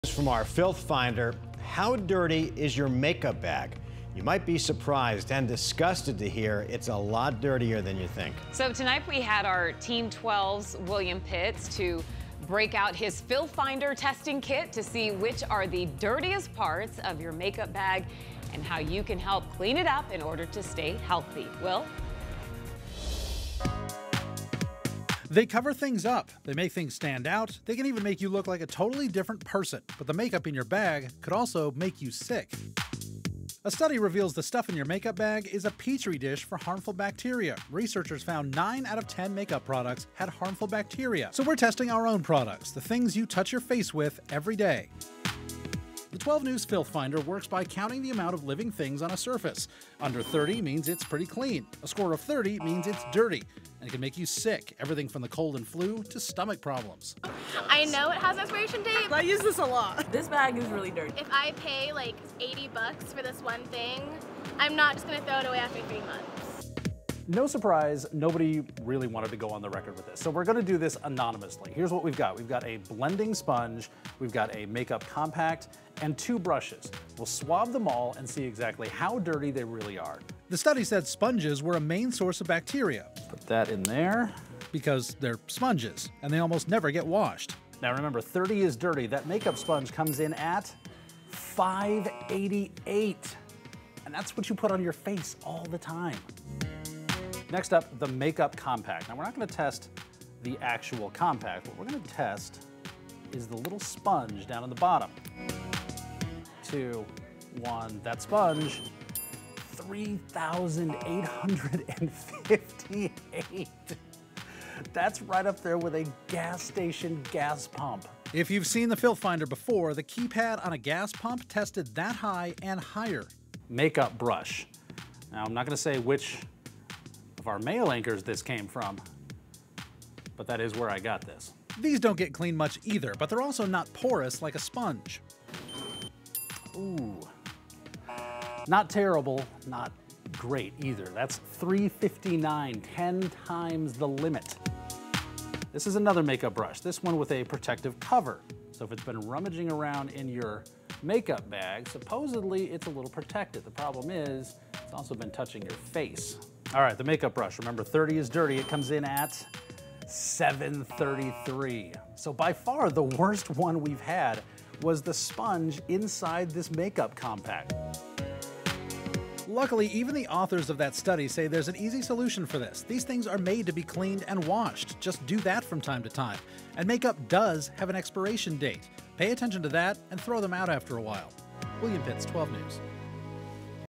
Filth Finder: How dirty is your makeup bag?
Will Pitts broke out the 12 News filth finder to see how many germs are hiding in various makeup products.